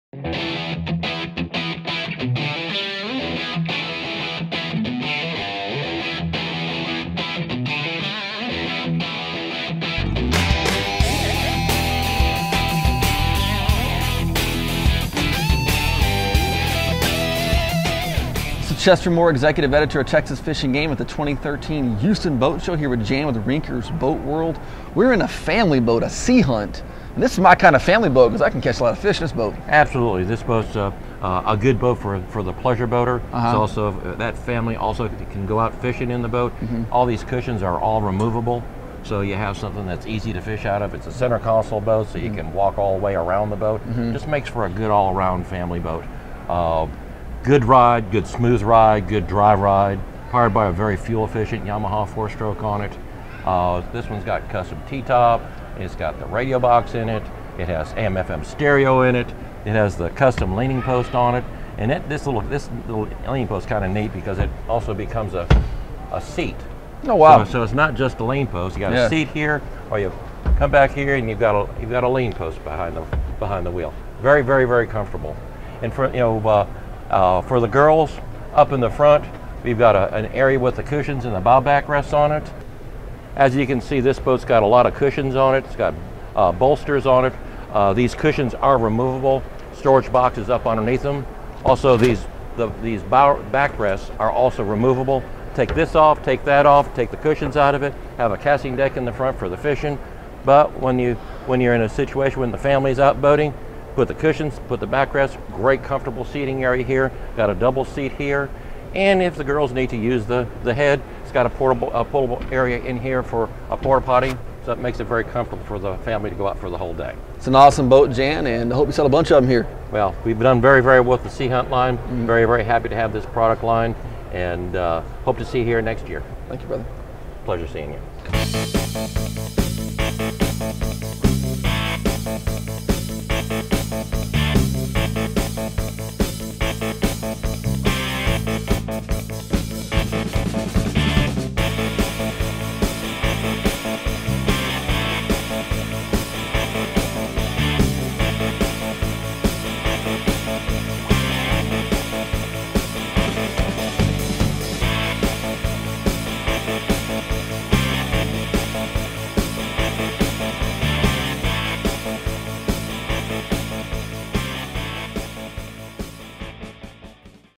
Rinkers Boat World: Sea Hunt Ultra 211
Rinker's Boat World has on display the Sea Hunt Ultra 211, literally the ultimate fishing boat for the family. ©Texas Fish & Game,